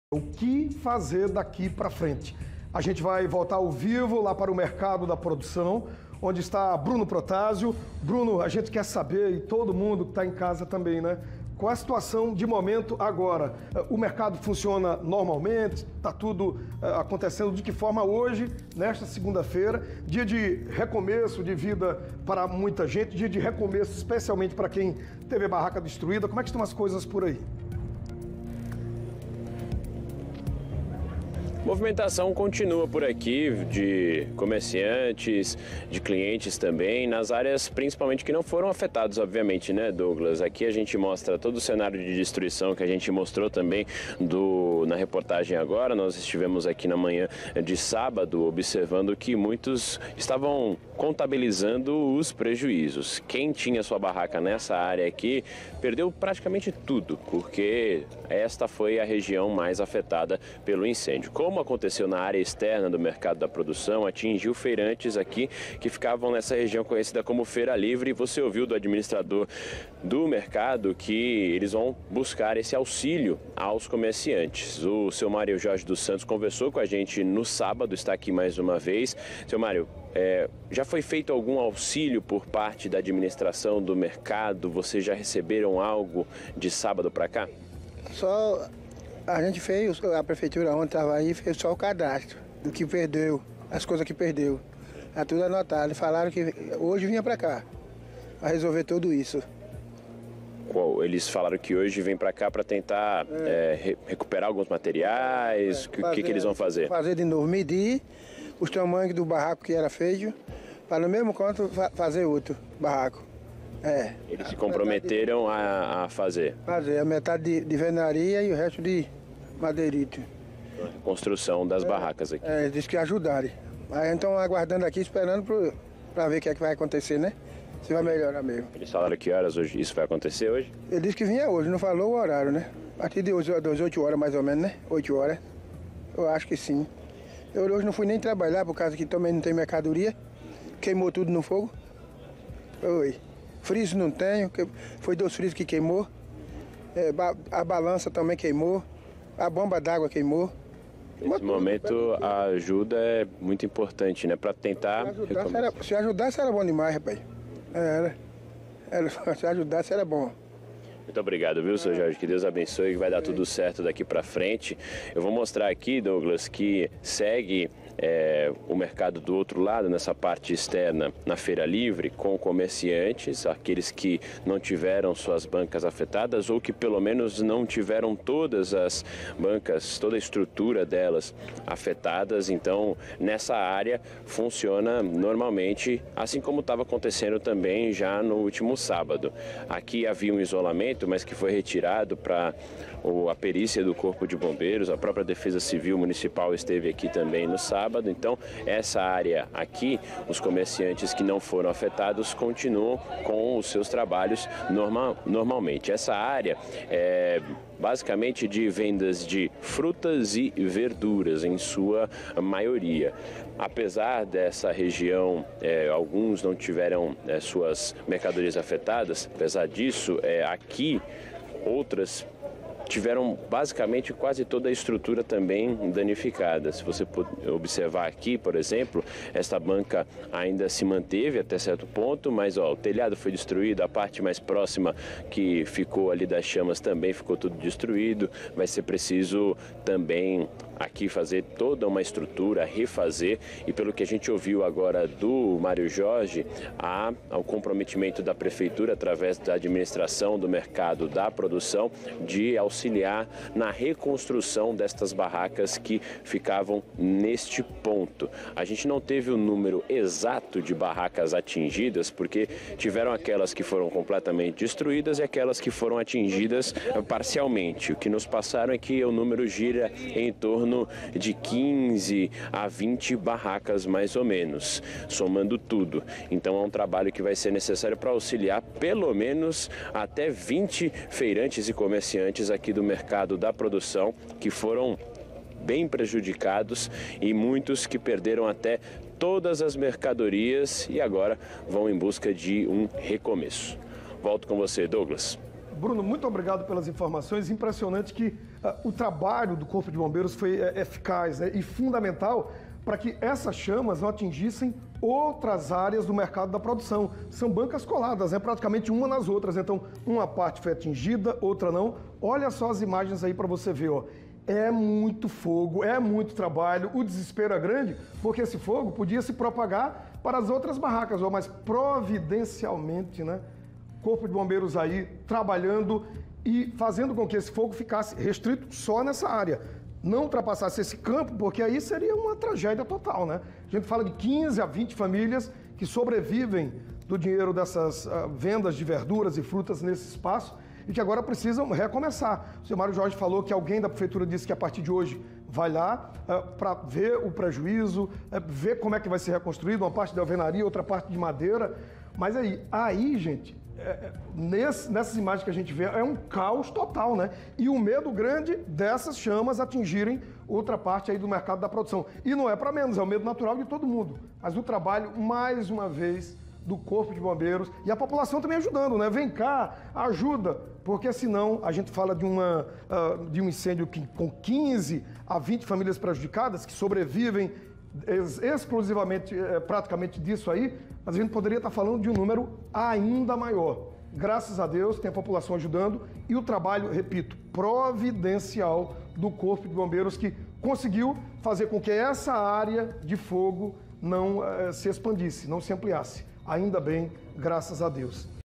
Incêndio da Levada: Mercado da Produção reaberto hoje
Veja como está o cenário de destruição.